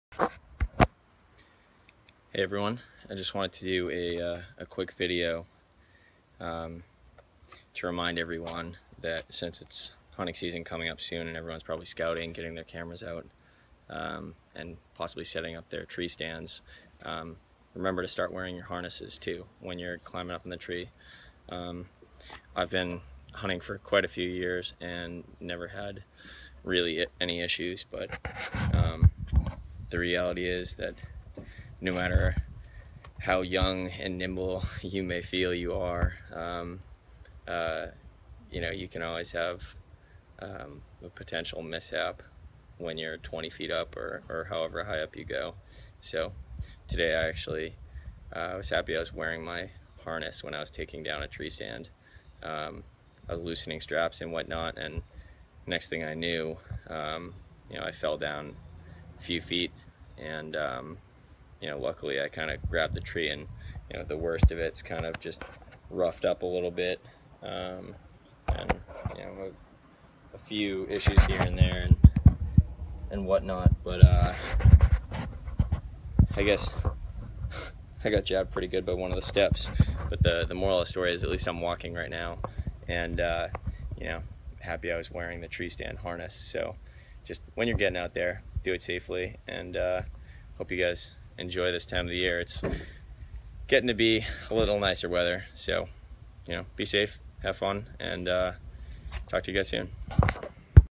Wear a safety harness!
I've been hunting for over a decade from a tree stand and have never fallen or truly had a close call while elevated, yet I've been using a tree stand safety harness for years...thank God I was wearing one today.

Always wear your safety harness when elevated.